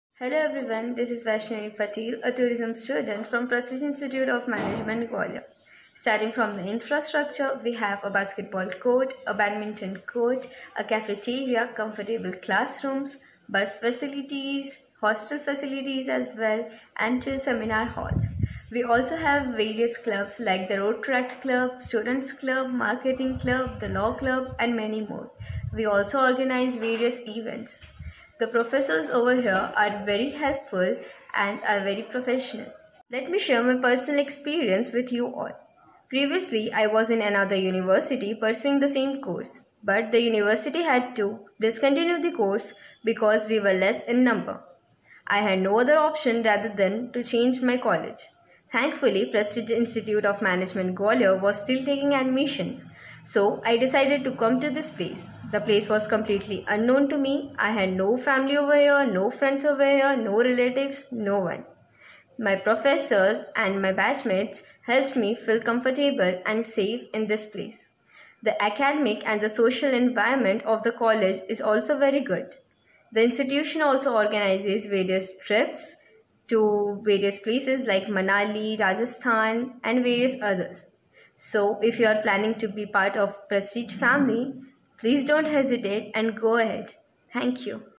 Bachelor of Tourism
If you want to learn about the features and logistics of BTM program at Prestige Institute of Management, Gwalior, Please see this video

Prestige Institute of Management, Gwalior